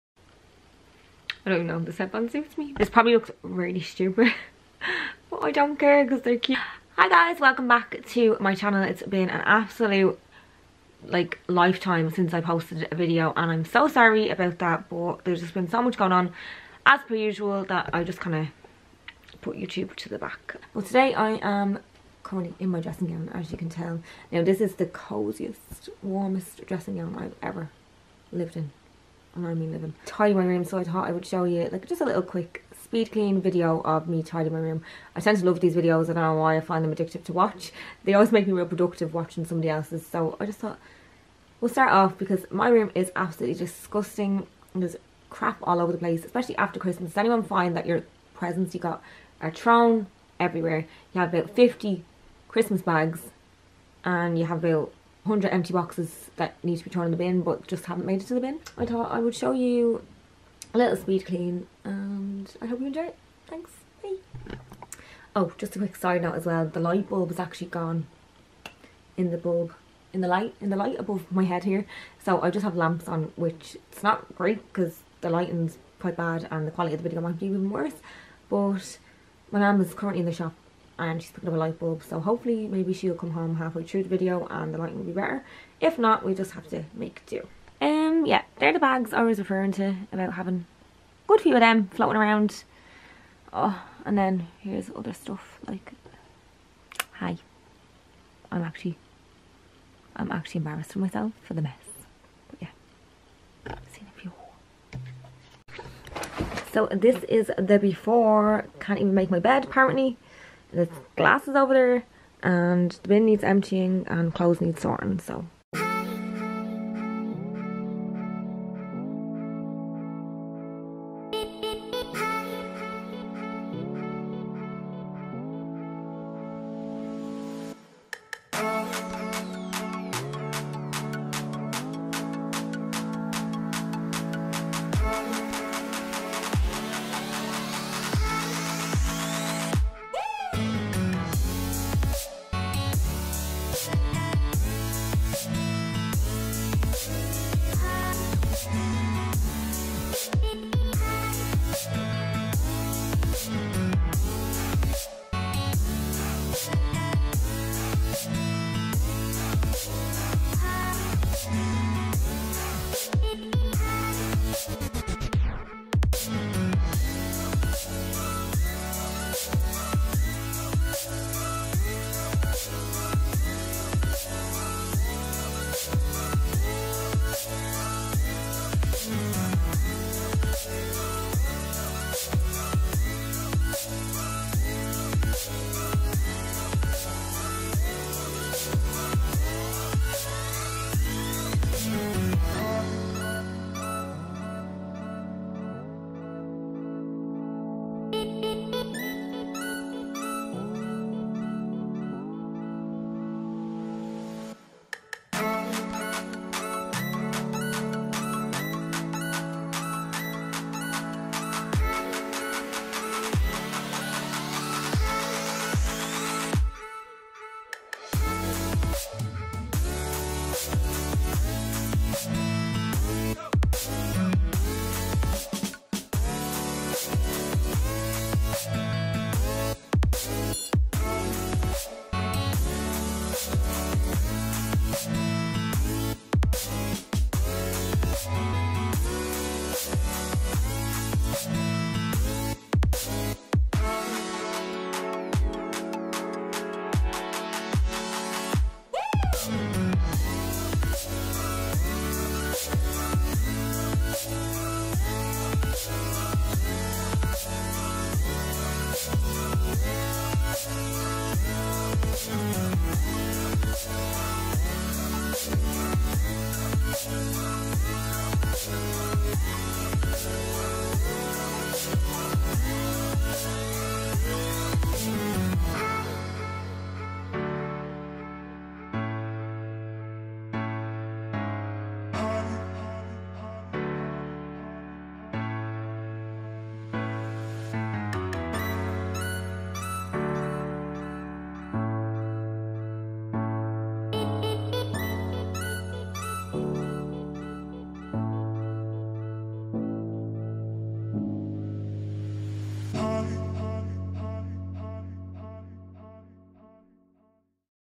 NEW YEAR SPEED CLEAN 2020: Extreme Cleaning Motivation/Messy Bedroom Transformation
2020 NEW YEAR SPEED CLEAN WITH ME/CLEANING MOTIVATION/MRS HINCH CLEANING HACKS 2019

Hi everyone, hope you all had a lovely christmas! I hope you enjoy the video & let me know if you would like more of these type of videos on my channel.